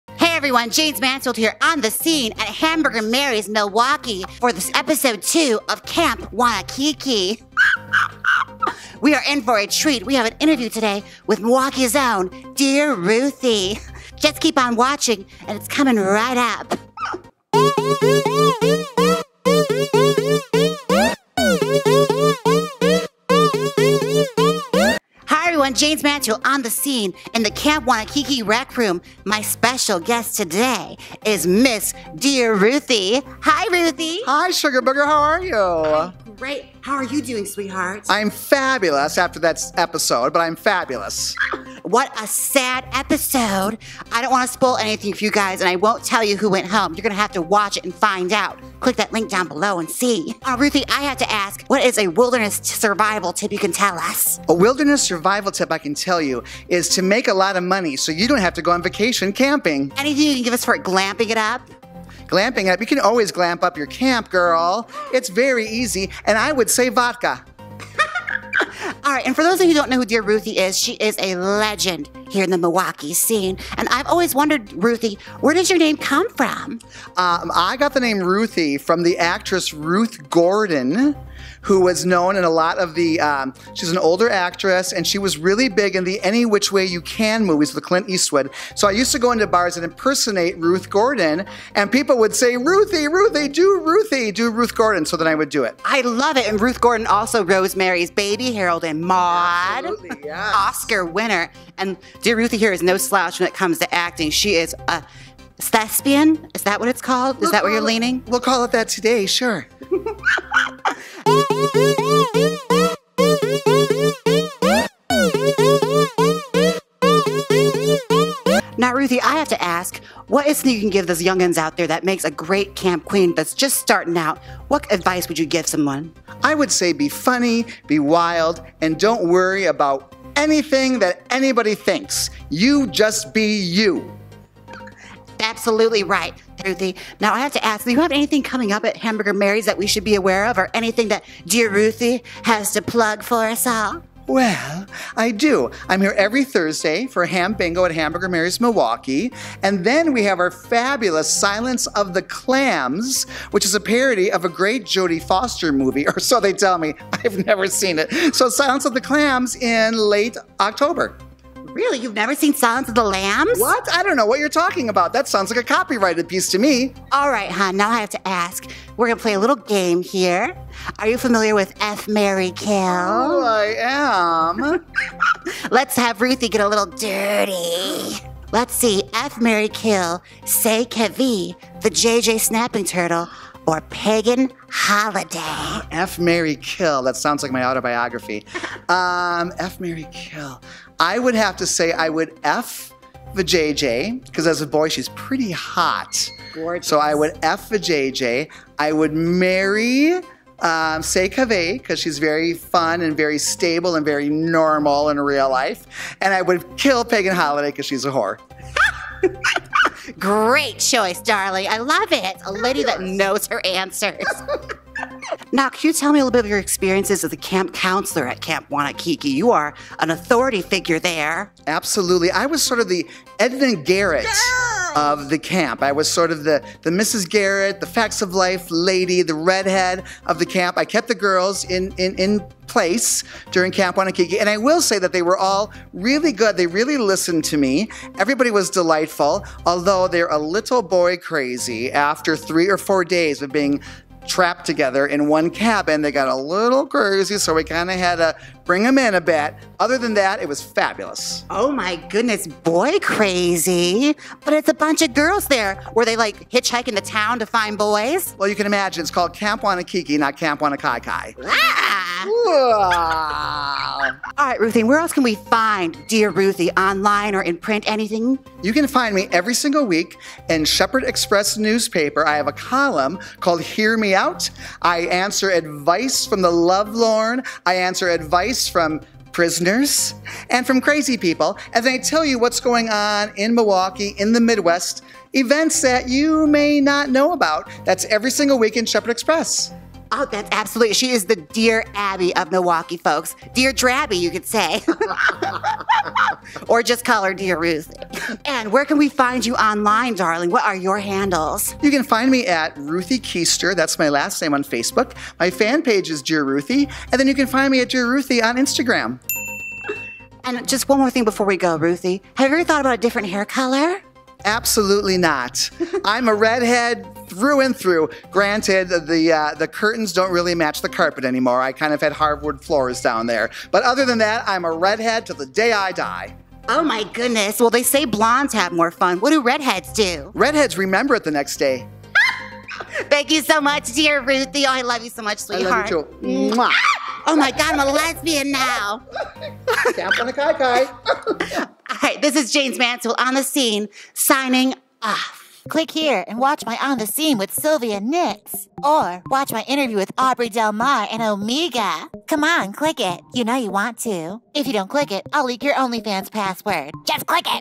ON THE SCENE | CAMP WANNAKIKI FEAT. DEAR RUTHIE | JAYMES MANSFIELD
Hi kittens in this On The Scene, we stopped by Hamburger Mary's Milwaukee for the second episode of Camp Wannakiki. I was delighted to sit down with Milwaukee's own Dear Ruthie, she's the head counselor at Camp Wannakiki and she's one tough cookie.

READ DEAR RUTHIE'S COLUMN HERE!

FILMED & EDITED BY: @ERNIEREYY & @JAYMESMANSFIELD